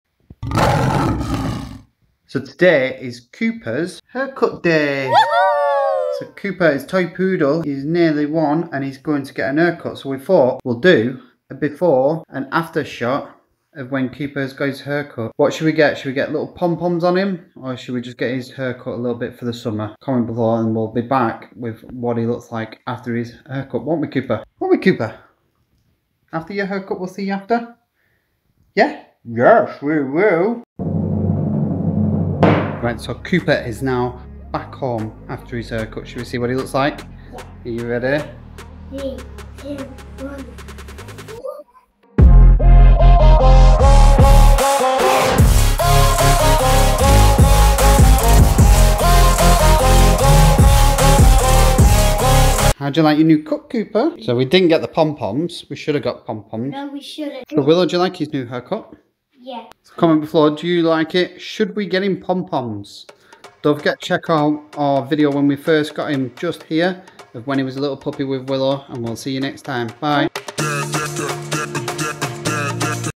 Toy POODLE Teddy Bear HAIRCUT 2022 (Toy poodle puppy)
Groom day for this Toy Poodle teddy bear when he gets a haircut, You will full in love with this
Toy poodle puppy